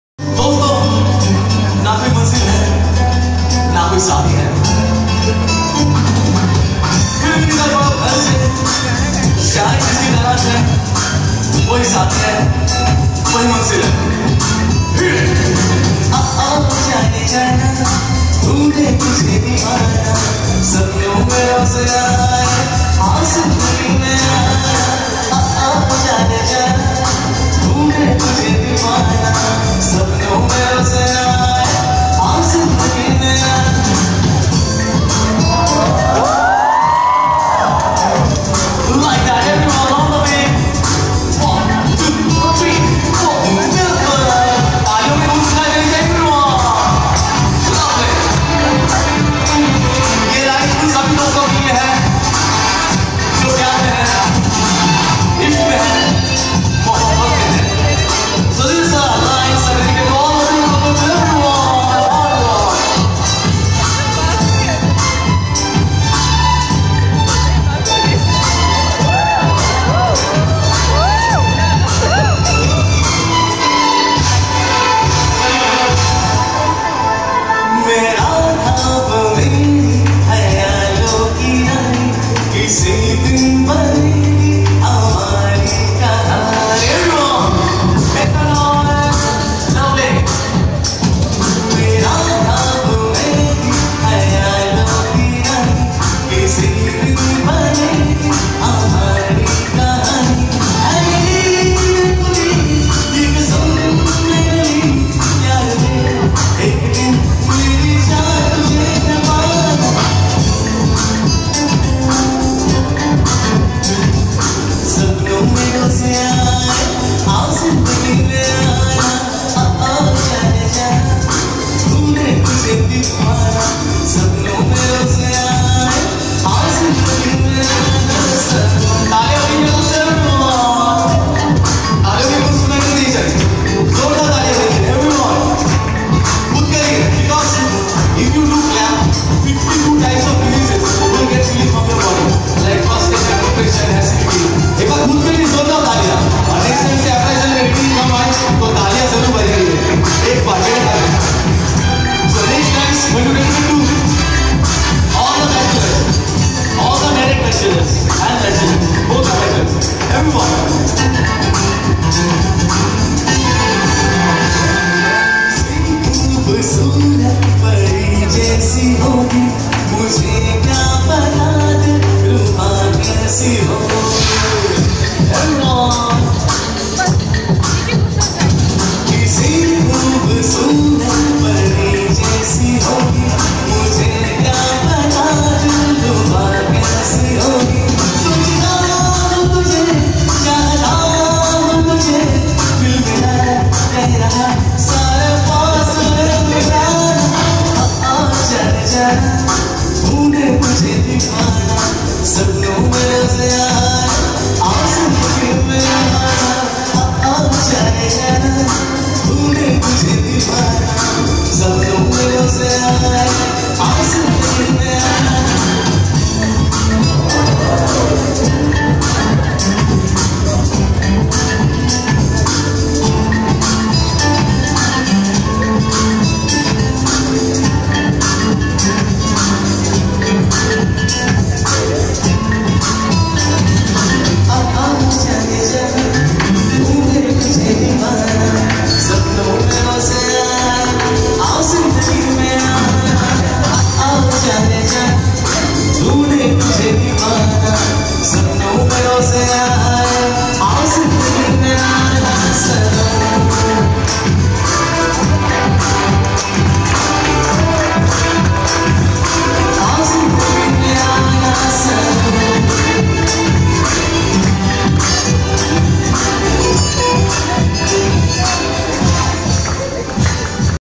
MY first TGIM show in Mumbai Capgemini campus being the part of Rubberband ~ Rick Saha
Singer ~ Rick Saha aka Rick D Performer
Event :- TGIM {Thank God Its Monday } 22nd April 2019 (My first TGIM performance in Mumbai Airoli ( CKP)